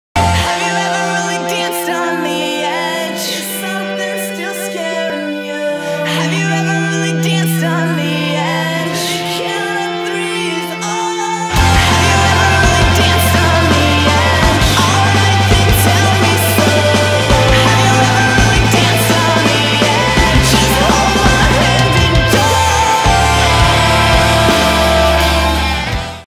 Just hold my hand && j u m p
Vent video. Feeling mehh lately, miss biscuit. I saw him last weeken which was great. He seems happy in his new home. Cubby has been fantabulous I love him so much. :) Yeah thats about whats going on in my life right now. Sparkle is perfect too!